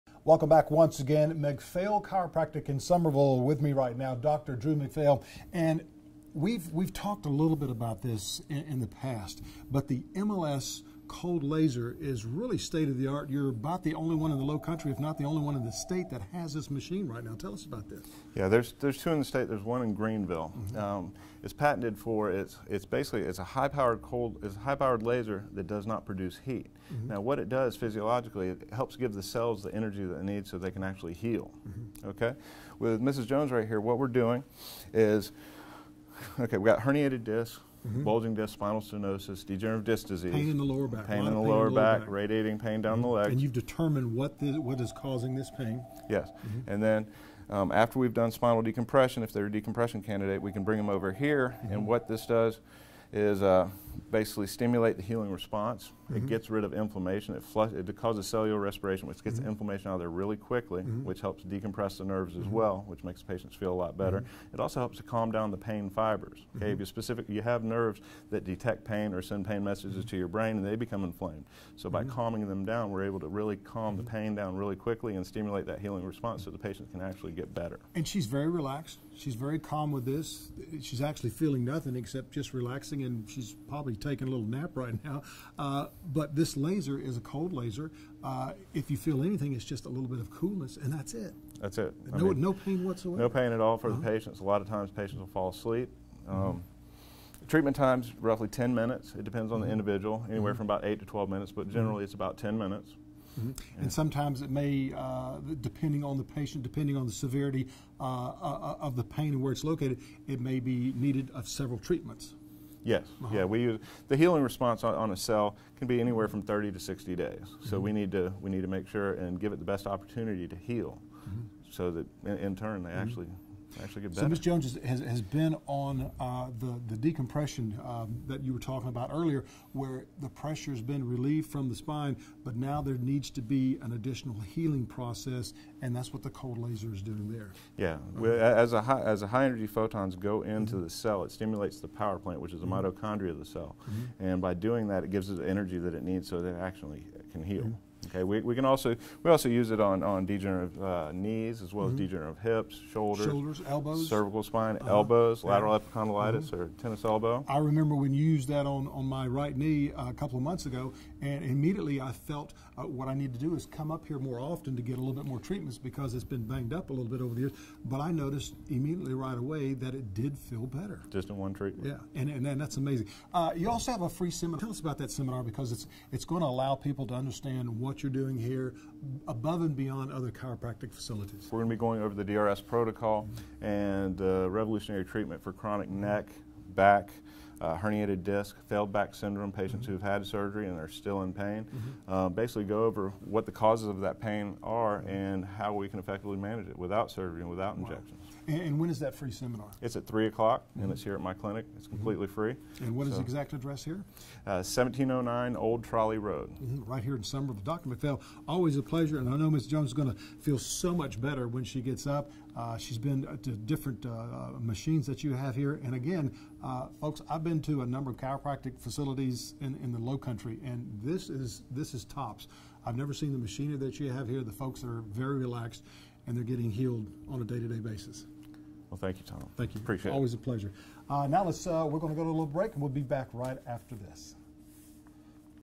Knee Pain Relief with cold laser at McPhail Chiropractic Pain Relief Center
We use a high powered cold laser, (MLS) that does not produce heat. This cold laser helps relieve neck pain, back pain, shoulder pain, elbow pain and/or hip pain. This laser helps remove inflammation, calms the nerve fibers and stimulates the healing response. Healing time varies based on the severity of the inflammation and/or injury. This treatment is used for degenerative knee, degenerative hip, degenerative shoulders, cervical spine issues, and/or lateral epicondylitis(tennis elbow).